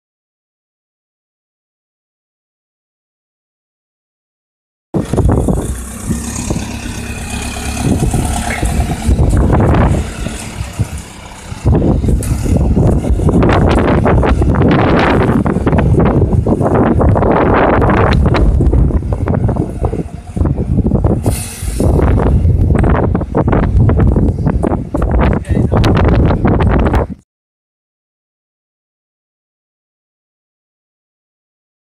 Captering G.H.A. Coaches Wright VDL Cadet YJ04 BVD
Using a different camera this time which is my phone called The Sony Xperia D2005
Which is Taken on the 28th of March 2015

And this phone which I use for my Flickr account